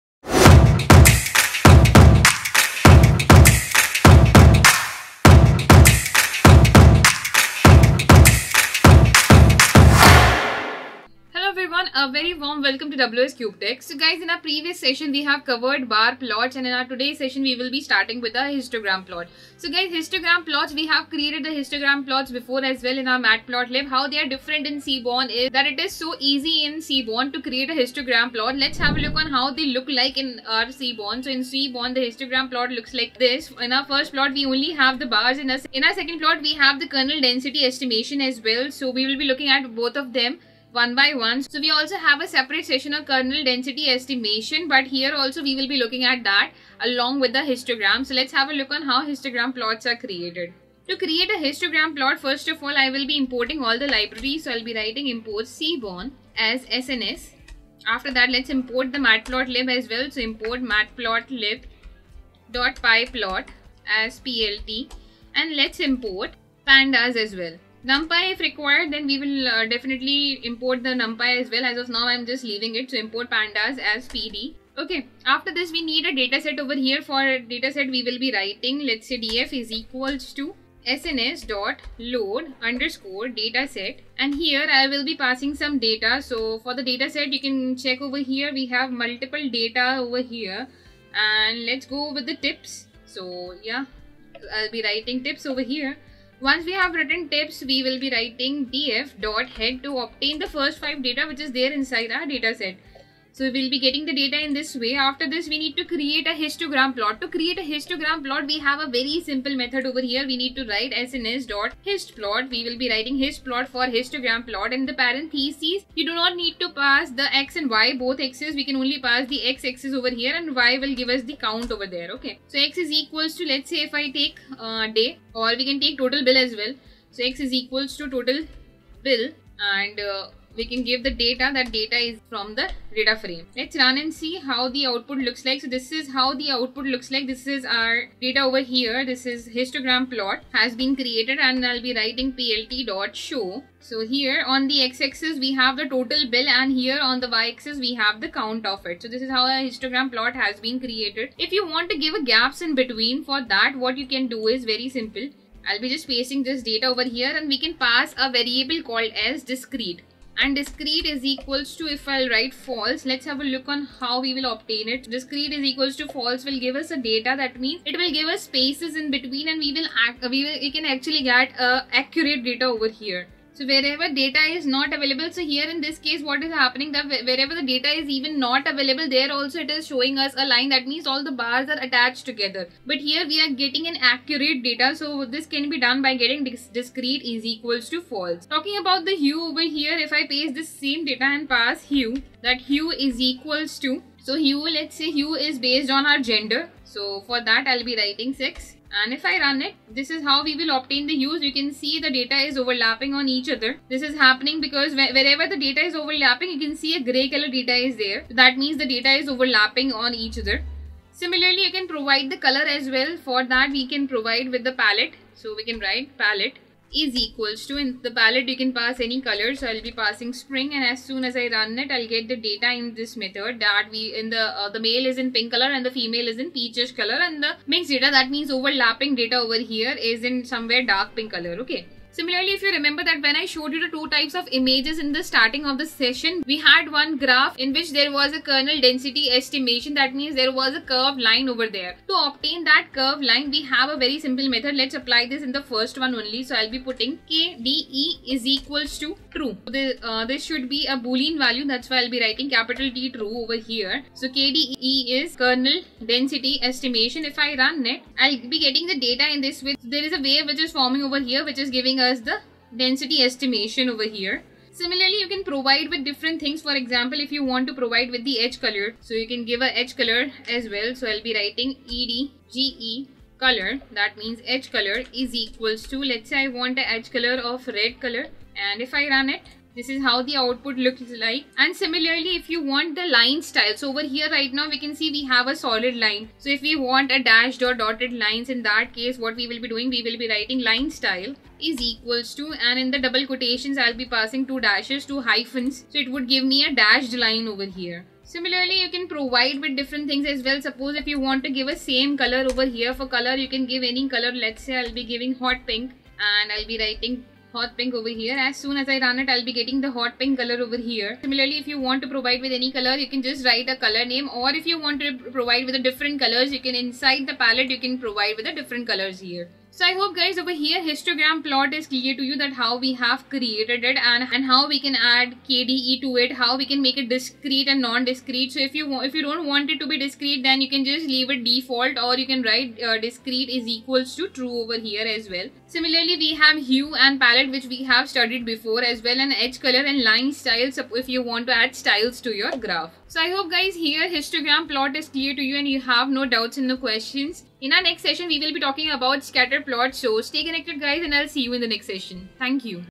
Seaborn Histogram | How to make a Seaborn histogram plot with Python code
In this video, learn Seaborn Histogram | How to make a Seaborn histogram plot with Python code. Find all the videos of the SEABORN Complete Tutorial for Beginner to Advanced in this playlist